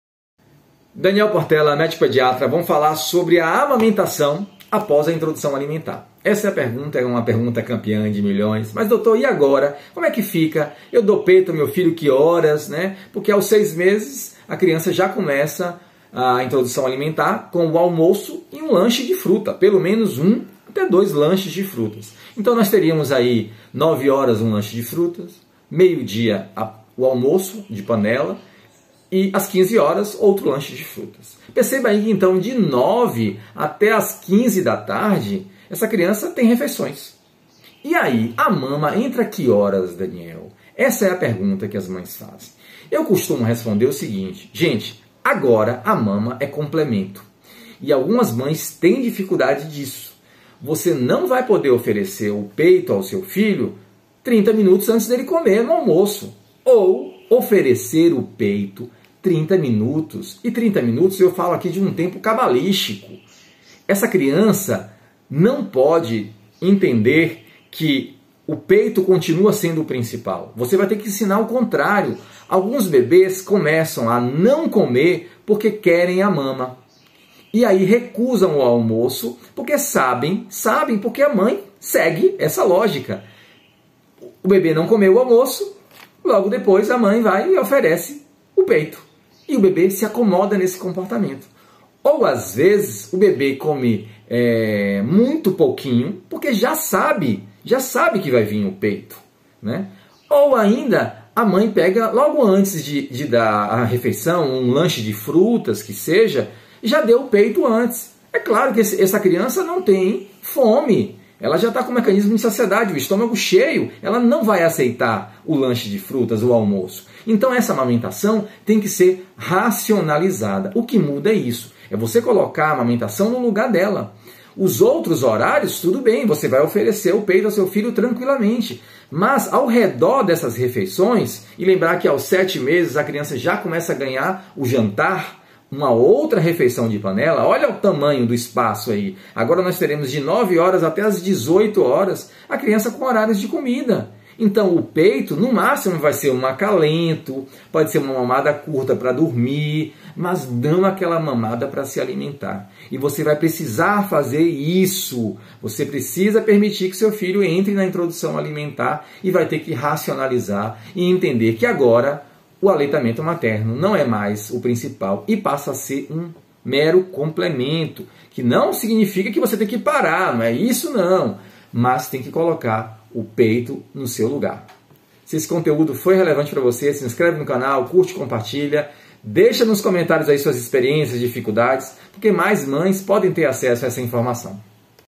Amamentação após introdução alimentar (IA): como proceder?
A amamentação apos a alimentação, como posso proceder?

CRM-BA 19663